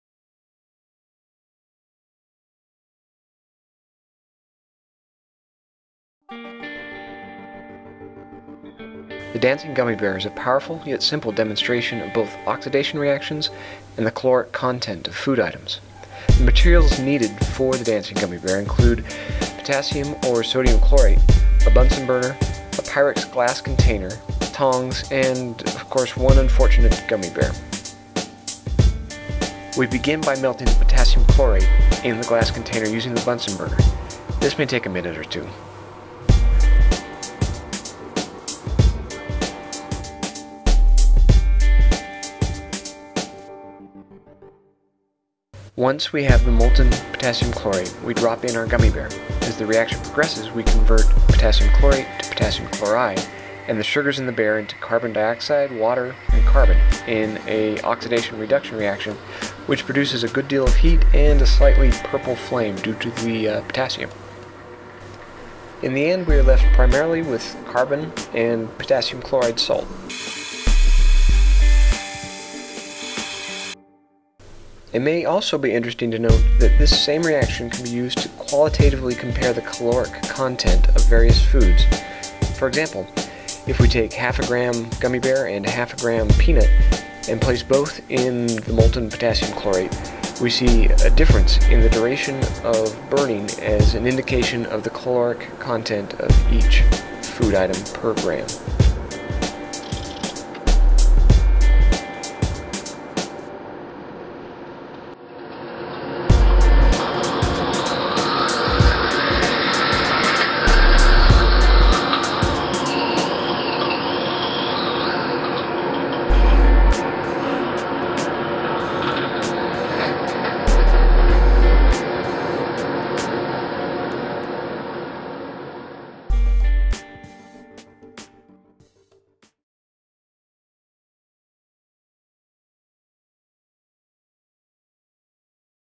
Dancing Gummy Bear - Outreach, Chem Eng, Univ of Utah
This demonstration displays the strength of Oxidation-Reduction reactions. When a gummy bear is mixed with molten potassium chlorate, its sugar breaks down into CO2 and H2O, while the potassium chlorate becomes potassium chloride. The reaction is vigorous and creates a great deal of heat and an impressive purple flame.